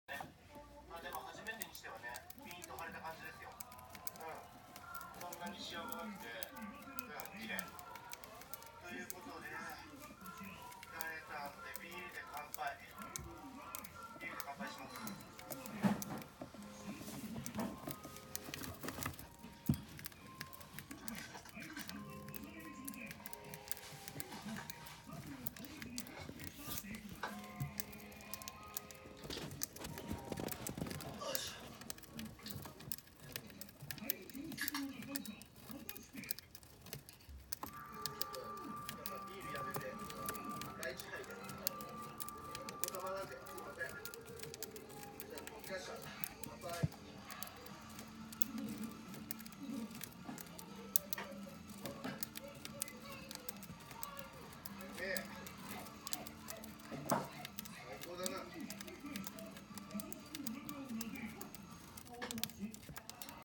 セキセイインコのサスケ
先程はサスケの爪を切ったので頑張ってくれたので大好物の赤穂の実を思う存分食べて貰ってます。粉っぽいのはネクトンを掛けてます。
栄養補助食です！笑